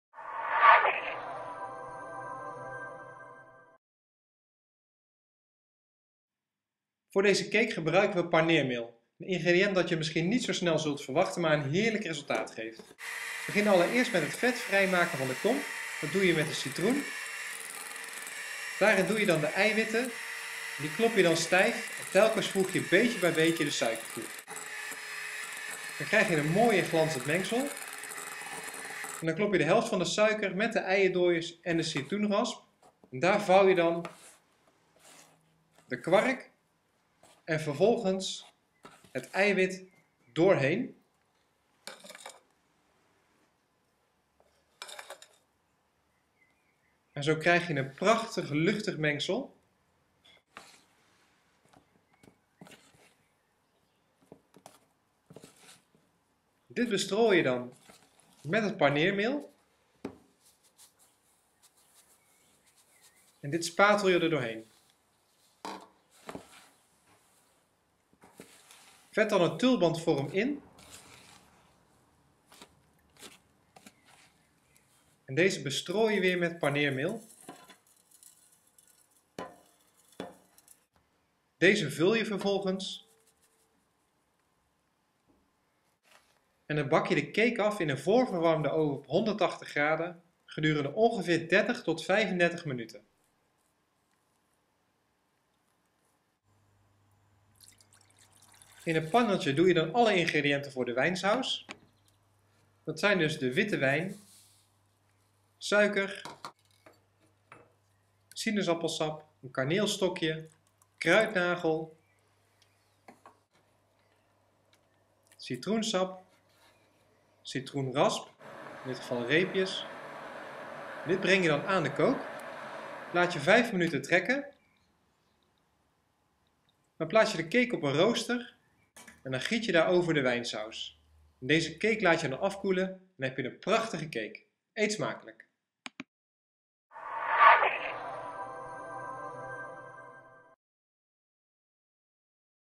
Cake met wijnsaus
Je zult het niet snel paneermeel toevoegen aan je cake. Maar in combinatie met de wijnsaus geeft dit een heerlijk resultaat. De ingredienten staan op